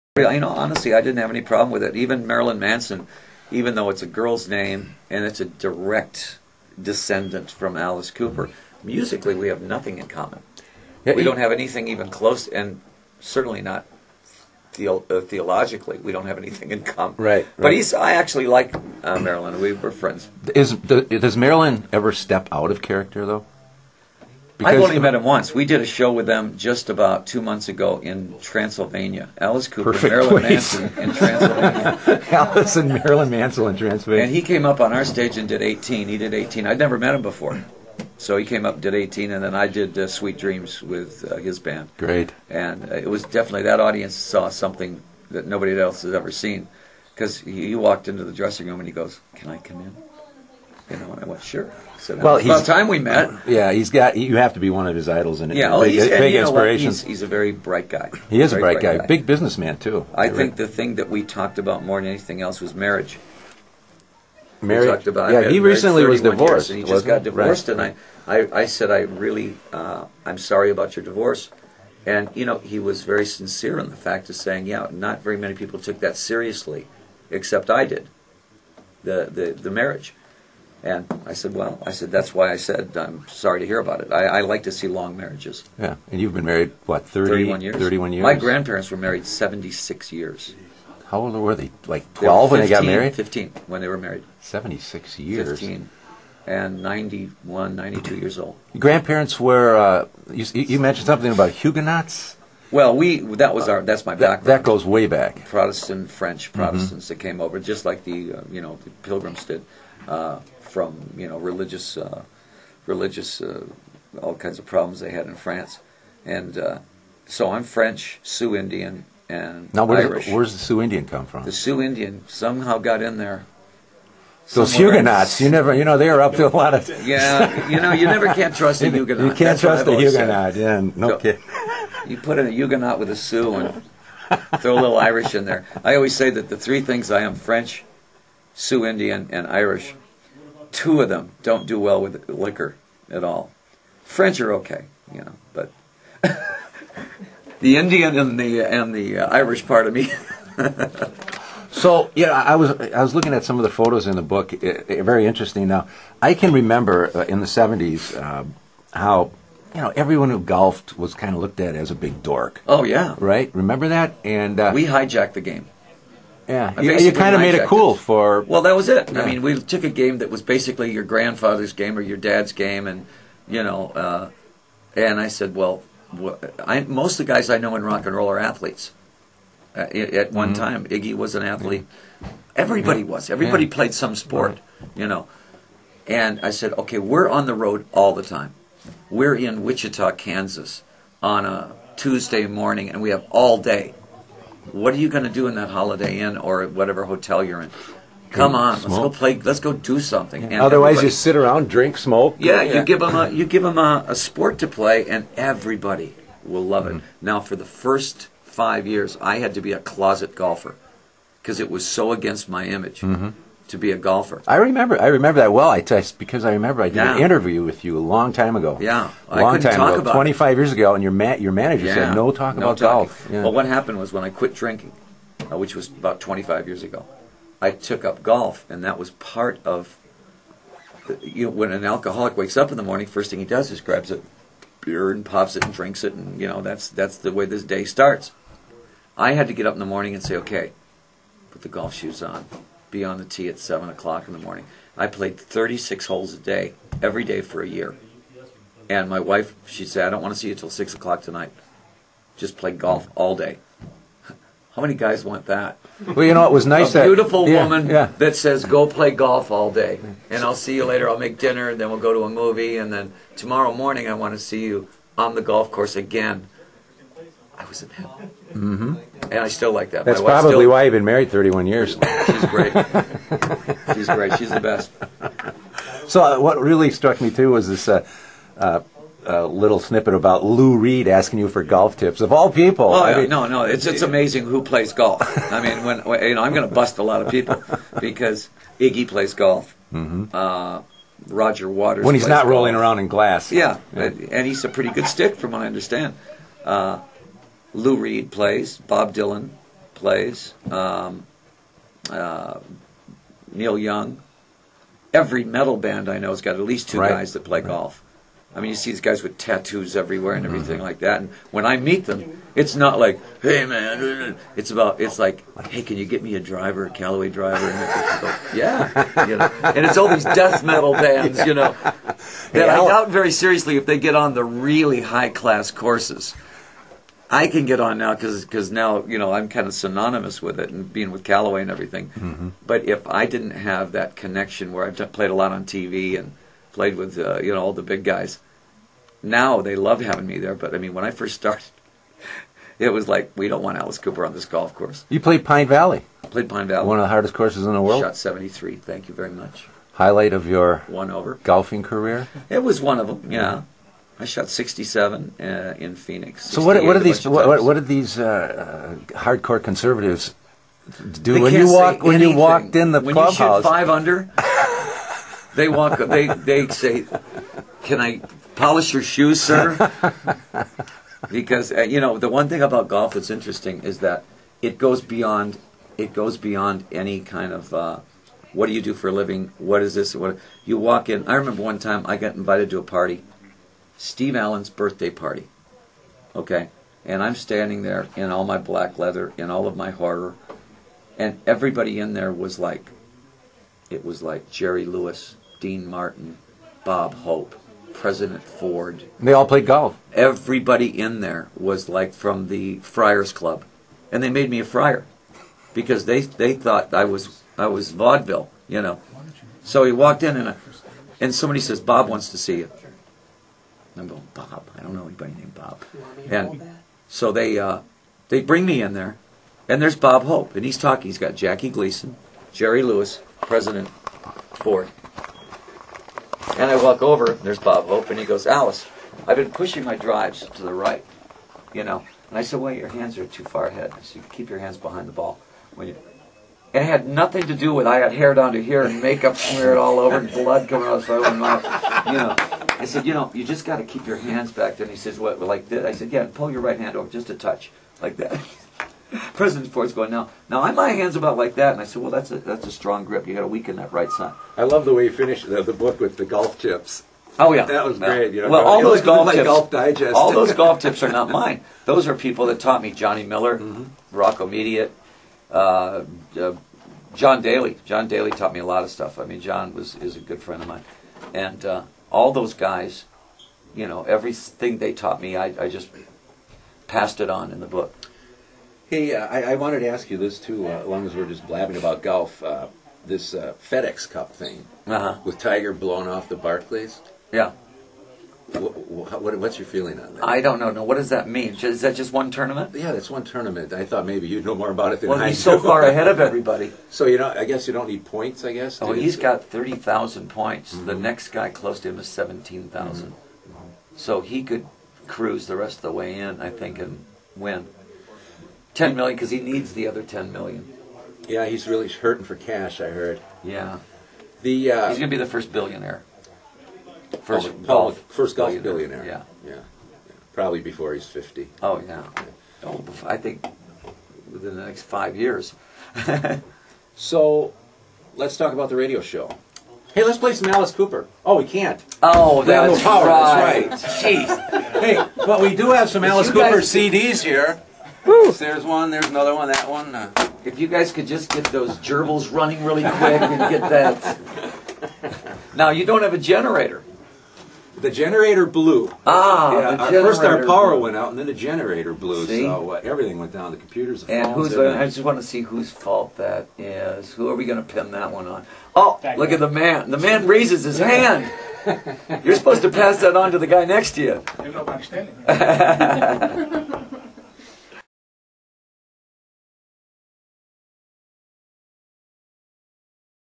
Downstairs Dan talks to Alice Cooper - part 2
WKLH's Downstairs Dan interviews Alice Cooper.

(note: As Alice entered the building the electricity and back-up generator died.  Weird.)